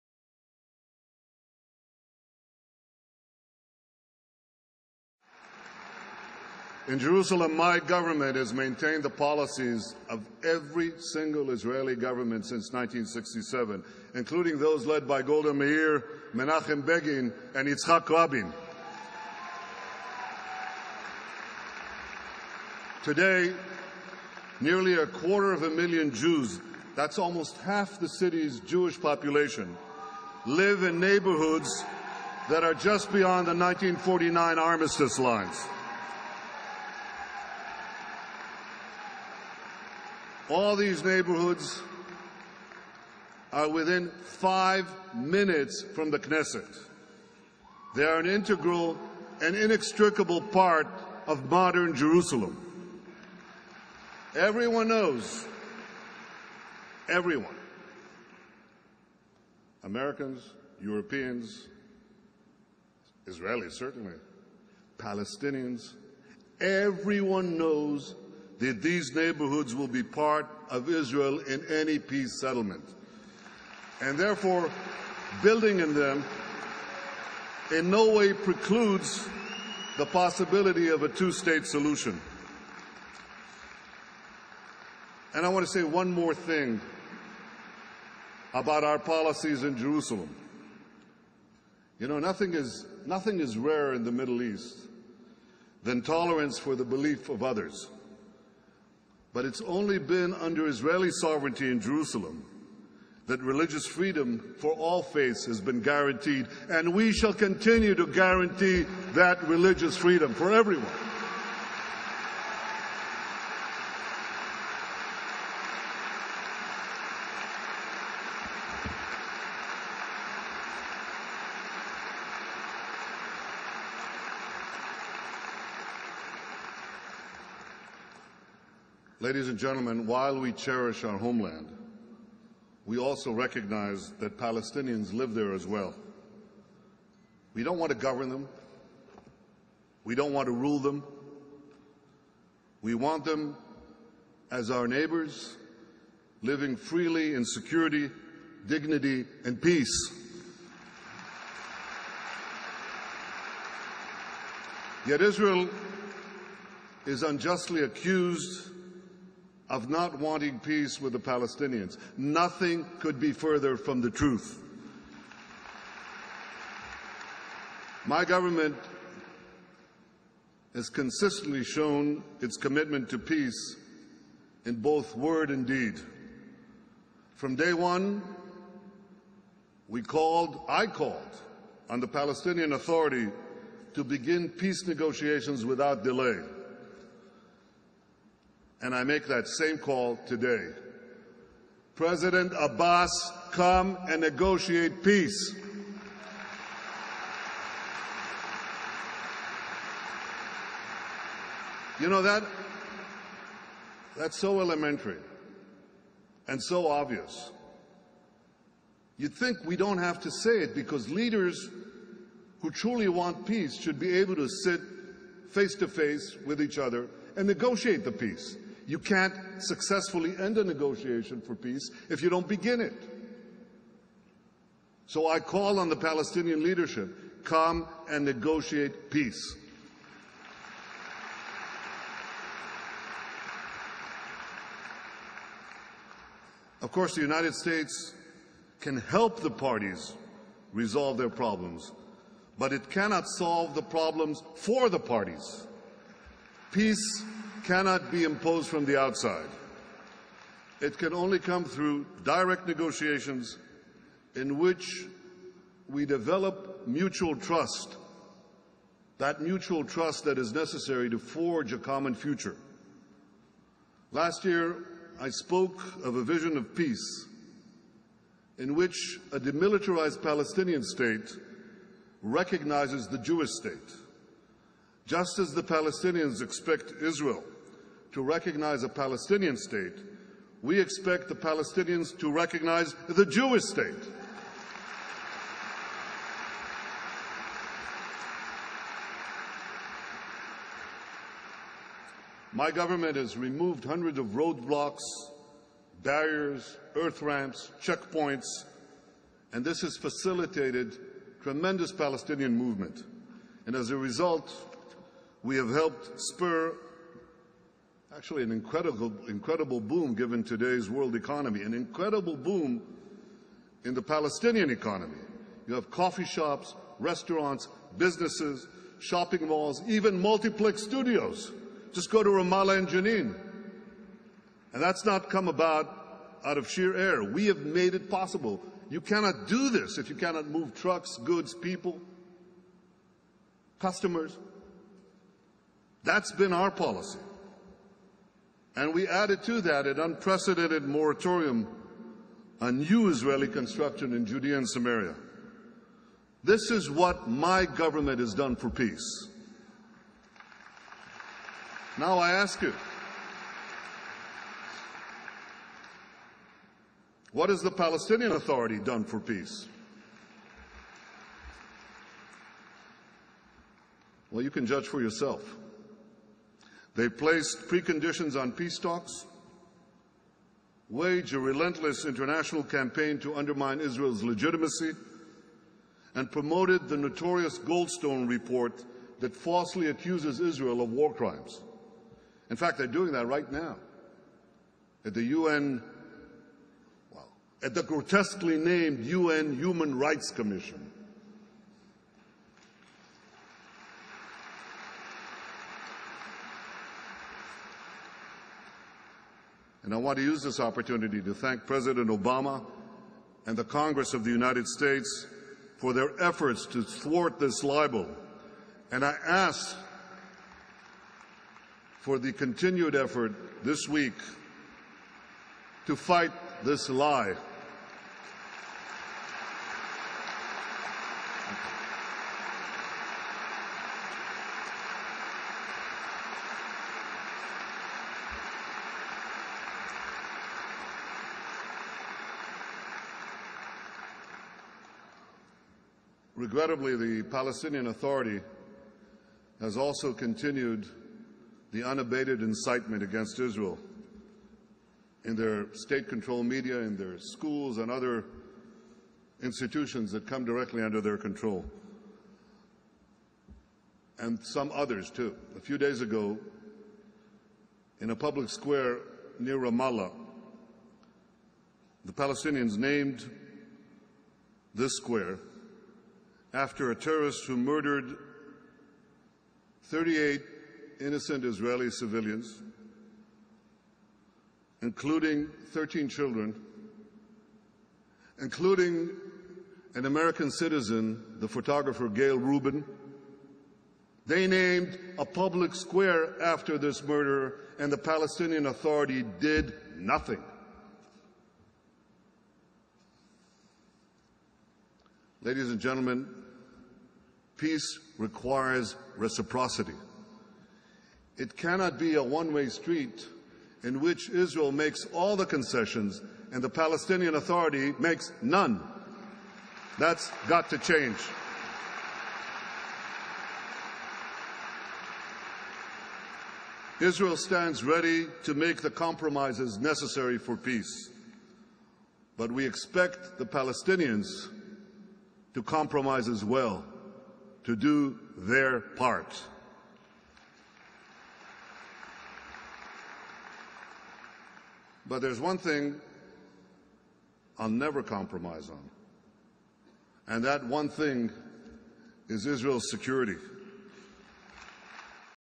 3ème partie du discours de Netanyahou devant l'AIPAC, le 22/3/2010
"Rien nest plus rare au Moyen-Orient que la tolérance envers les croyances des autres, mais ce n'est qu'avec la souveraineté israélienne sur Jérusalem que la liberté religieuse a été pour la 1ère fois garantie pour toutes les religions et nous continuerons à garantir cette liberté religieuse pour tous."
"Les dirigeants qui veulent vraiment la paix, devraient être en mesure de sasseoir en face lun de lautre et de négocier la paix. il est impossible de conclure avec succès une négociation de paix, si on ne la commence pas."
"Nous avons contribué à favoriser un incroyable essor de l'économie palestinienne, vous pouvez y trouver des cafés, des restaurants, des commerces, des centres commerciaux, même des cinémas multiplex. Allez donc voir à Ramallah et Djénine, et cela ne s'est pas fait tout seul par miracle, nous avons rendu cela possible."
"Il est regrettable que l'Autorité palestinienne continue sans relâche l'incitation contre Israël au niveau de ses médias contrôlés par l'État, dans ses écoles et autres institutions qui sont directement sous son contrôle"
"Israël est prêt à faire les compromis nécessaires pour la paix, mais nous attendons que les Palestiniens fassent aussi des compromis, quils fassent leur part."